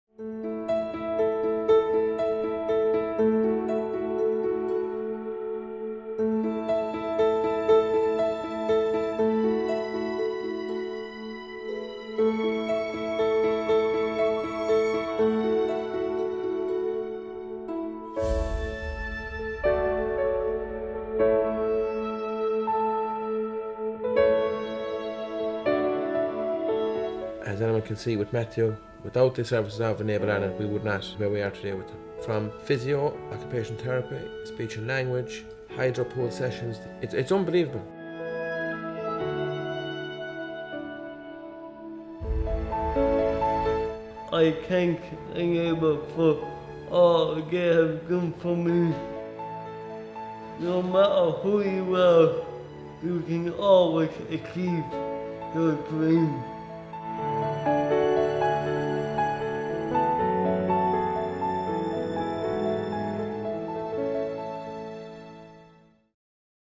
Introducing Enable Ireland
If you've never been introduced to Enable Ireland before then this is the video for you! Who we are summed up in one minute